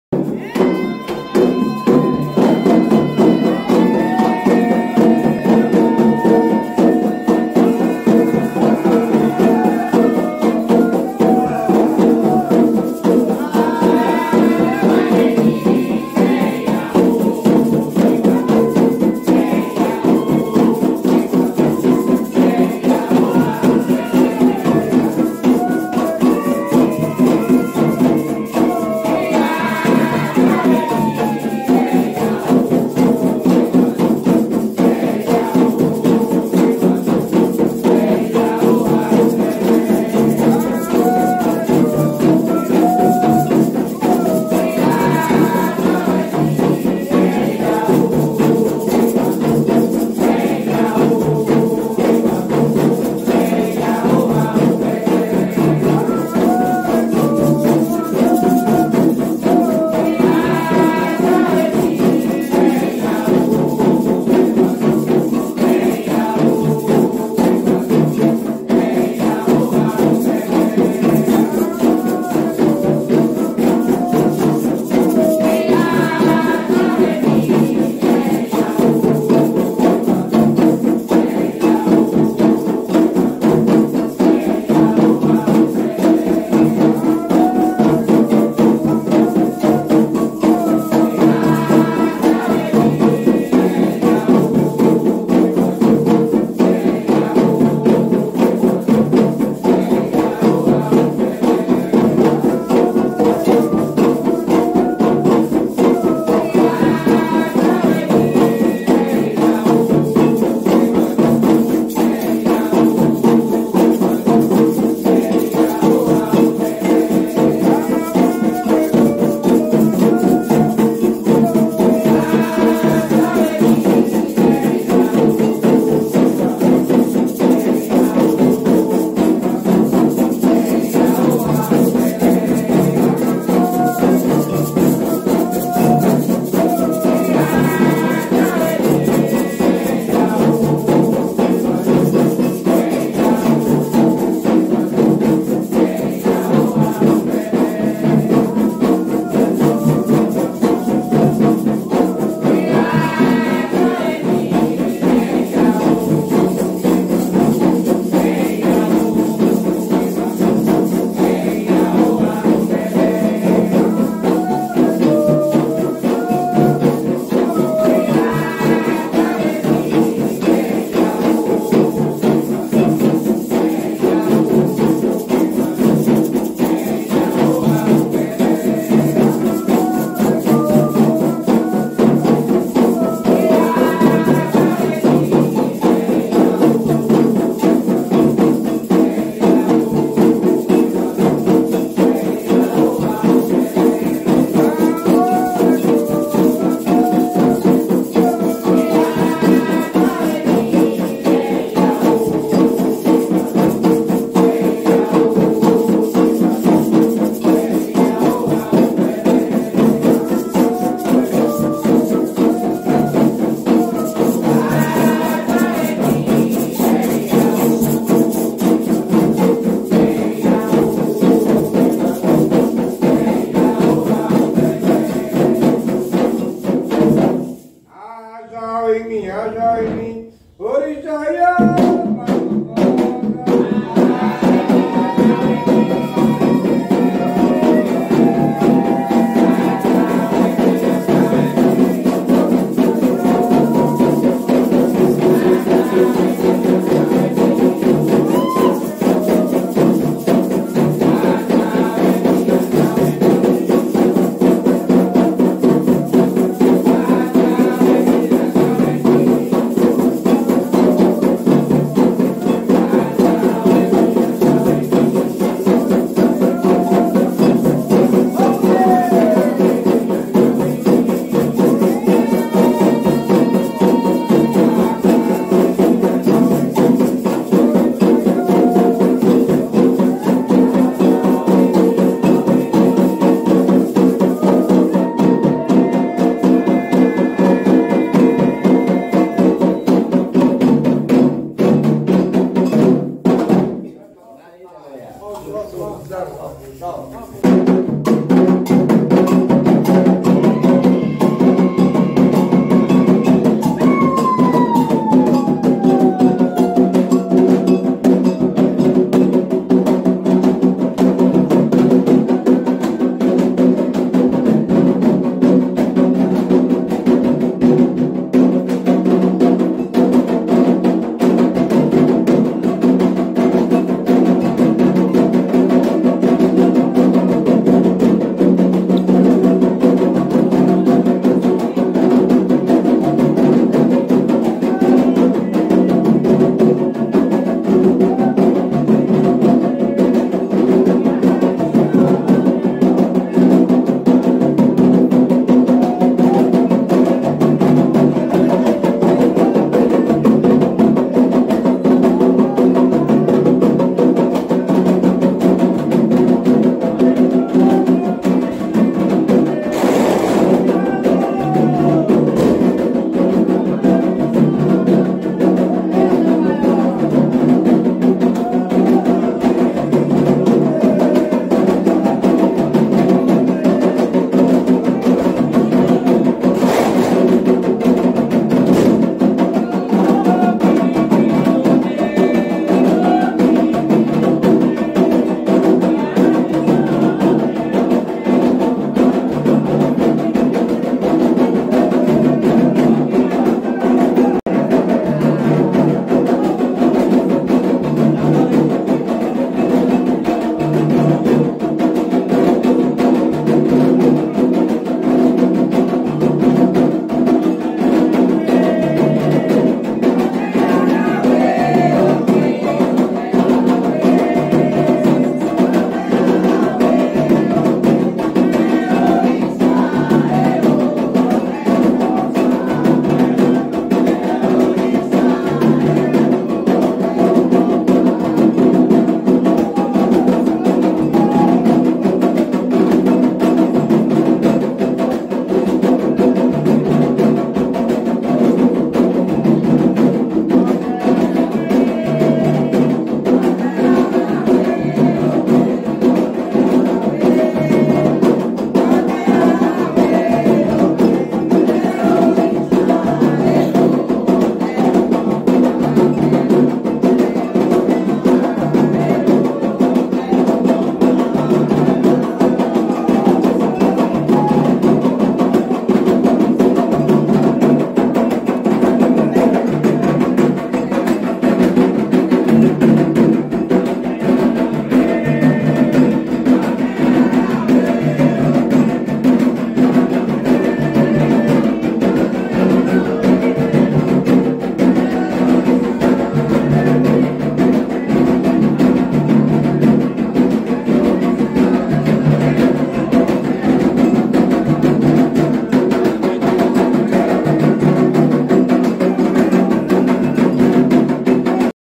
Candomblé - Puxada de Odú Ketá - Ajô de Neto Malta, Confirmação e Odú Ketá e Oyê de Marvin de Mejê.
Em 12/06/2022, em Riachuelo, Sergipe o Canal Candomblé Sergipano do YouTube com seu ADM Ogan Tony Ojuertan registrou seu louvor 231 no Centro Afro Jacuntá de Saára sob a coordenação do Babalórisà Eraldo Alves realizou o Ajô anual de Ogum Lárúndejí (Netto Malta), Confirmação do Alagbé de Ogum Robson Guilherme de Ogum Já e o Odum Ketá e Oyê de Marvin de Ogum Meje. com a quantia simbólica de R$ 2,99 (menos que um pastel) e assista antes os Xirés Completos e vídeos exclusivos. Candomblé Sergipano 13 anos louvando nosso Sagrado com você.